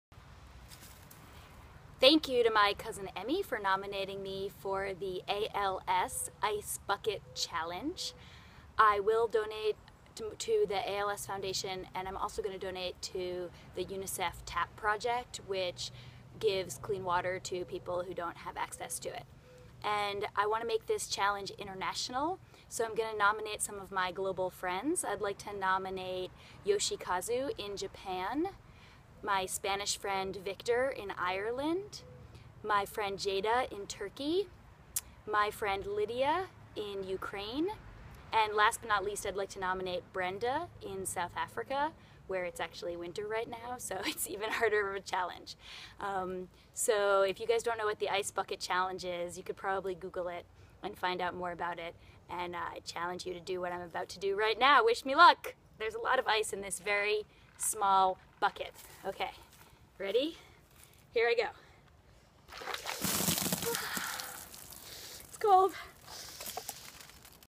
Making the Ice Bucket Challenge Global
I'm nominating friends in Japan, Ukraine, Ireland, Turkey, and South Africa!